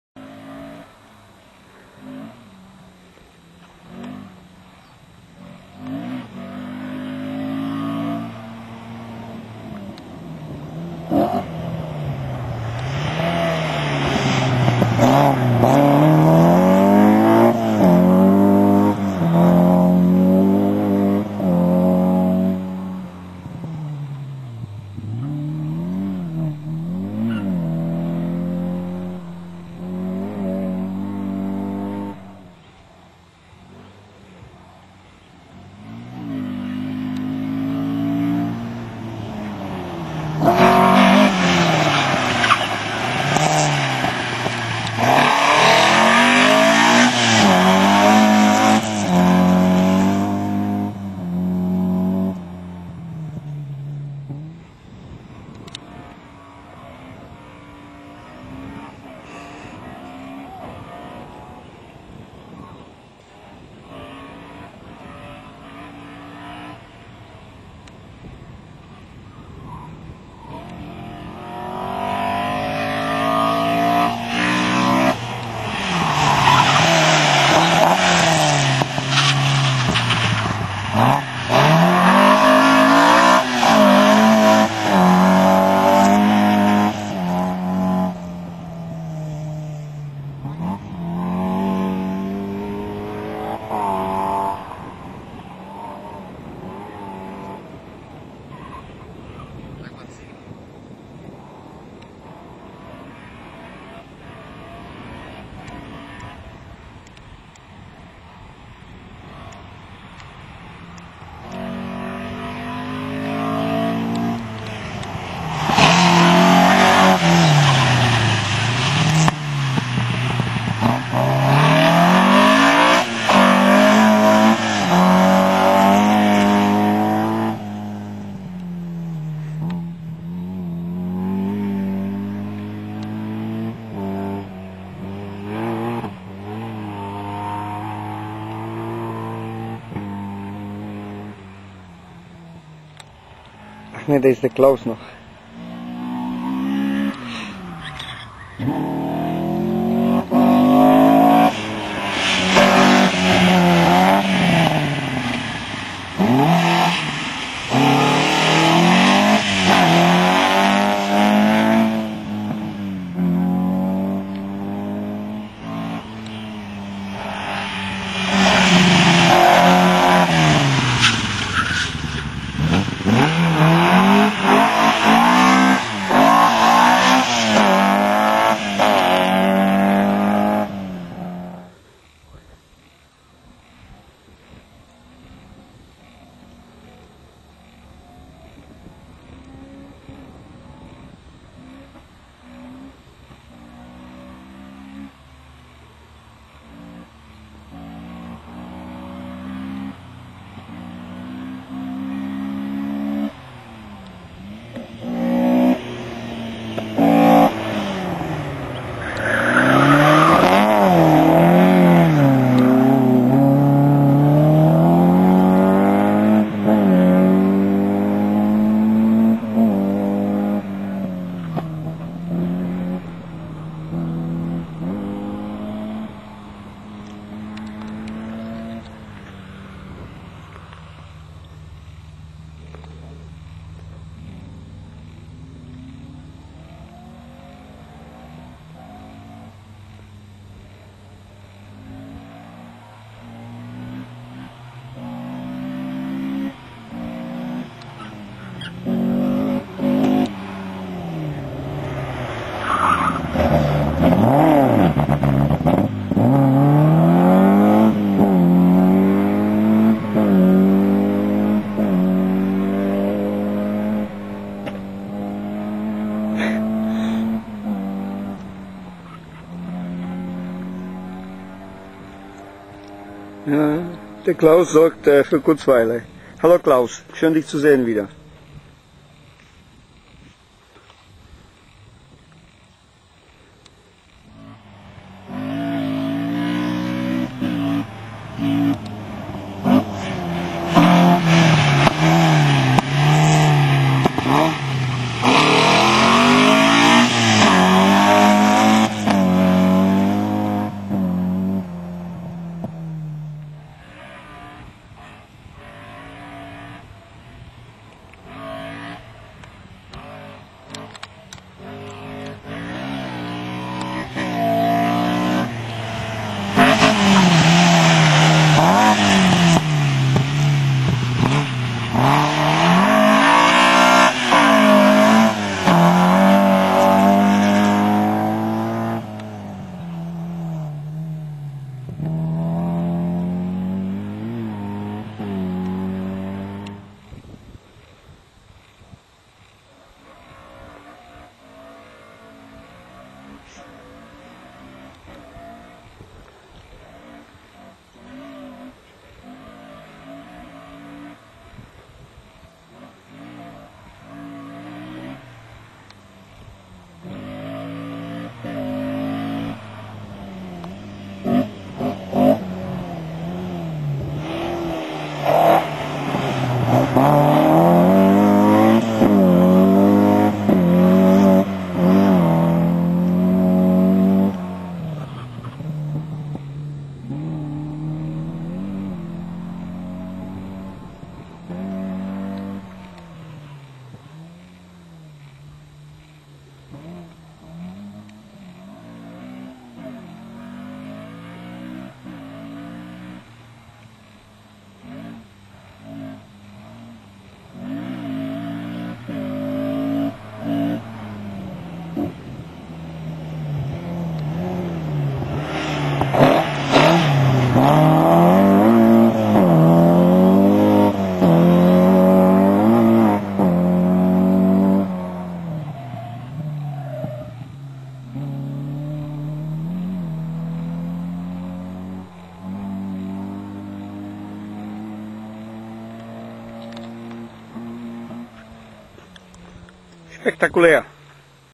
2009 Meschede DMSB Auto Slalom Klaus Euteneuer G. Katsikis Daniel Euteneuer Opel Kadett C / CQP
nach langer Zeit sind sie wieder da, beim Slalom : Klaus & "Katsi". Sie haben sogar Nachwuchs mitgebracht - den Sohn von Klaus, Daniel !!! Ich hoffe, wir sehen uns jetzt häufiger !!! :)))